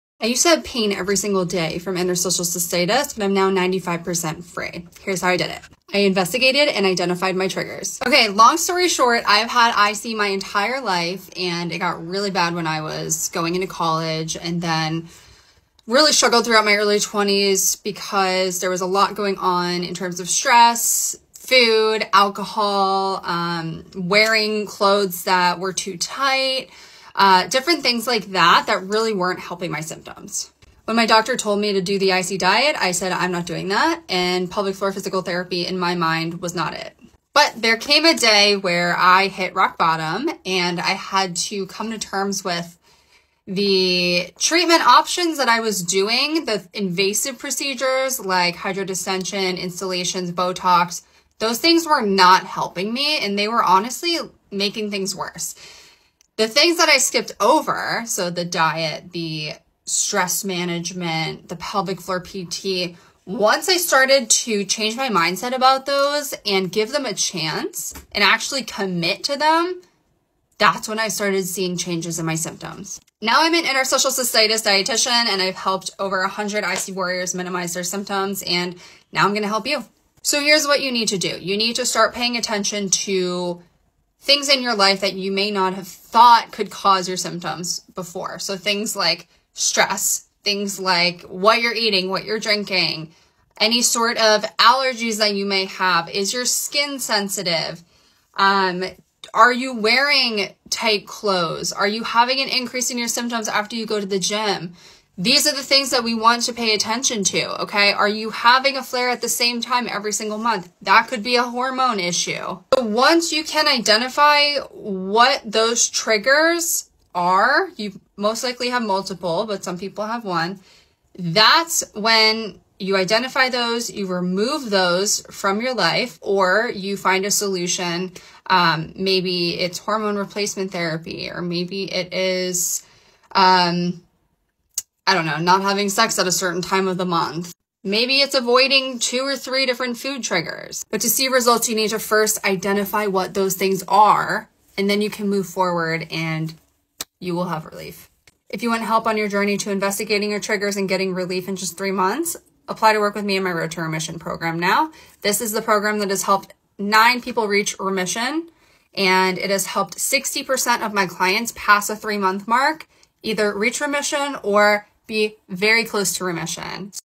How I managed my interstitial cystitis symptoms
How I managed my interstitial cystitis symptoms and am now 95% pain free